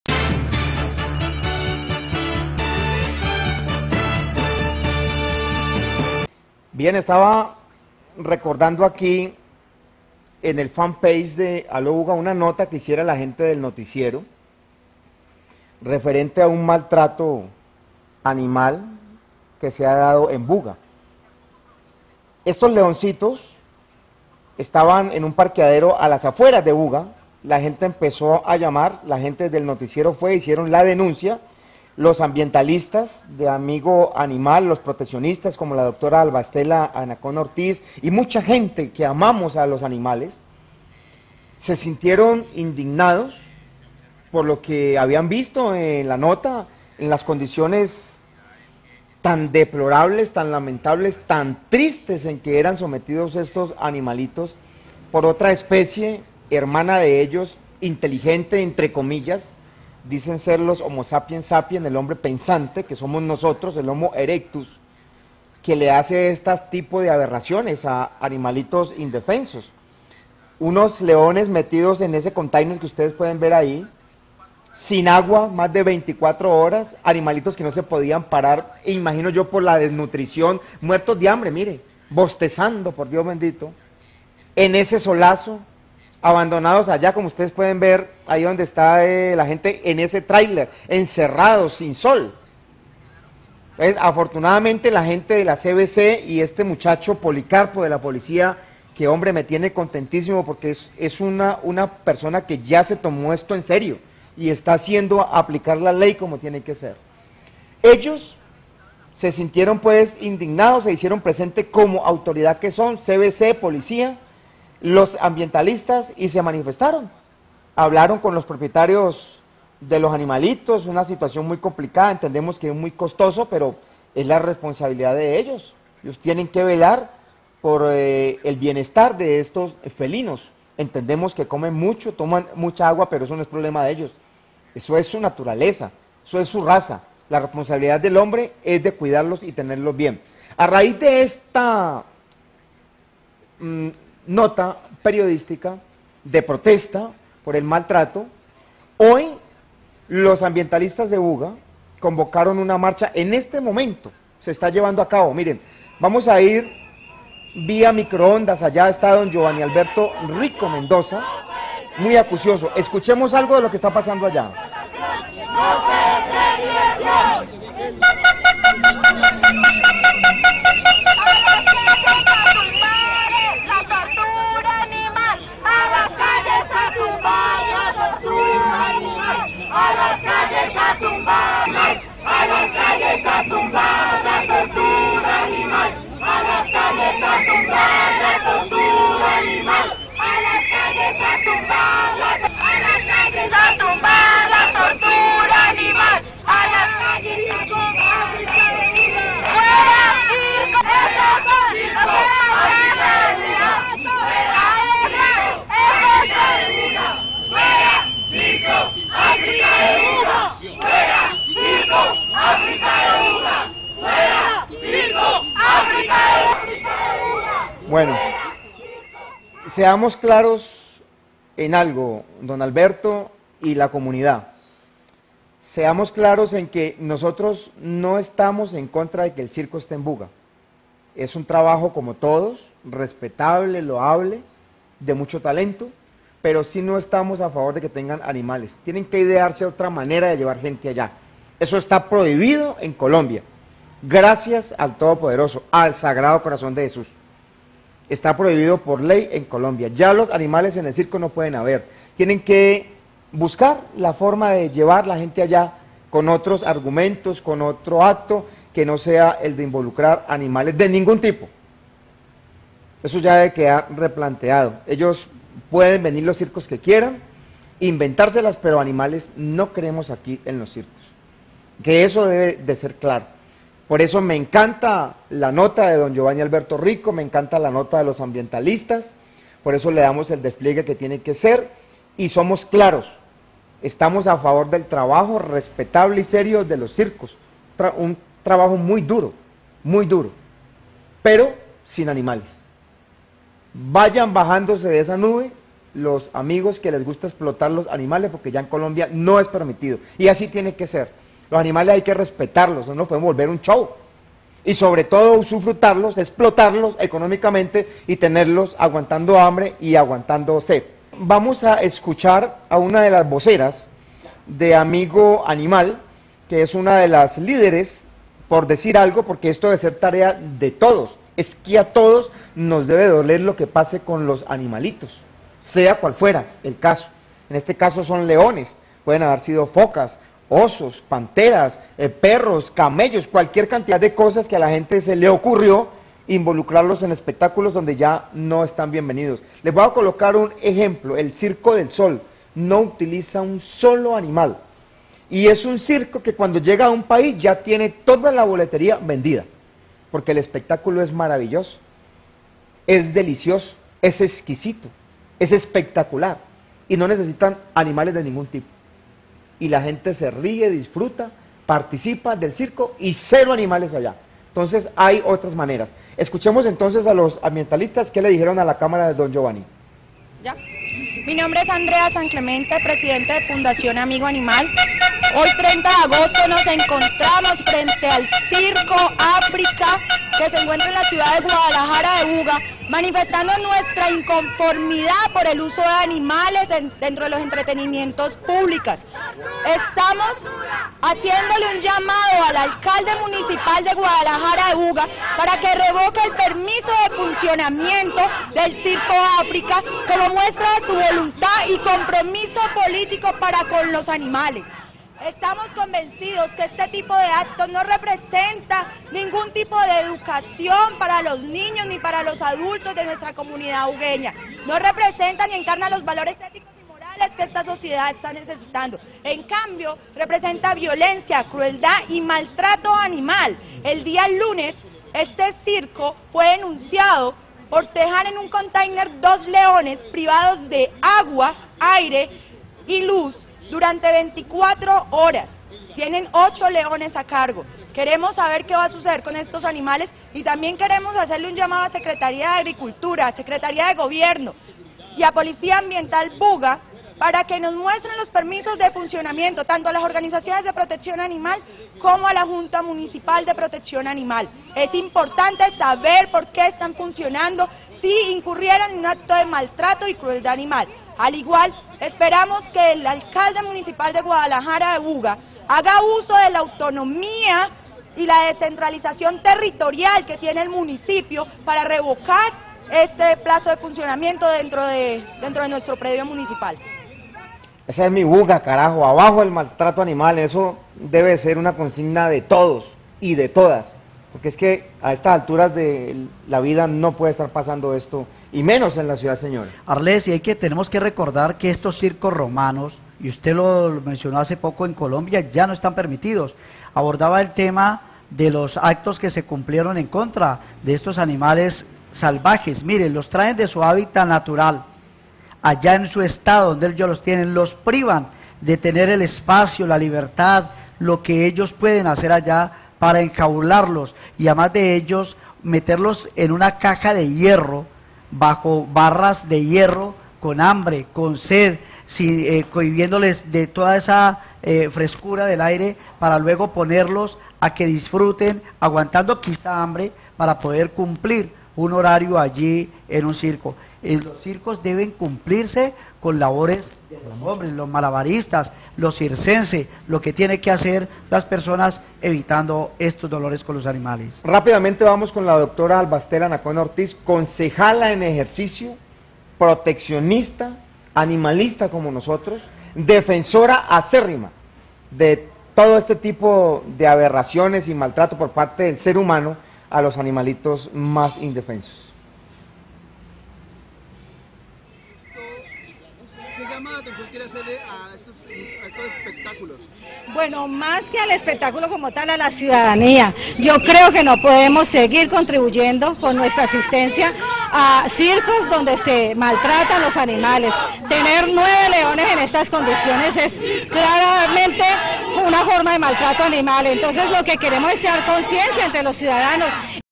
Protesta en Buga por arribo de Circo Africa, 2013
Activistas por los derechos de los animales protestan en Buga por comprobado maltrato animal en circo itinerante. "Circo Africa"
Entrevista Aló Buga Agosto 30 de 2013
Buga - Valle del Cauca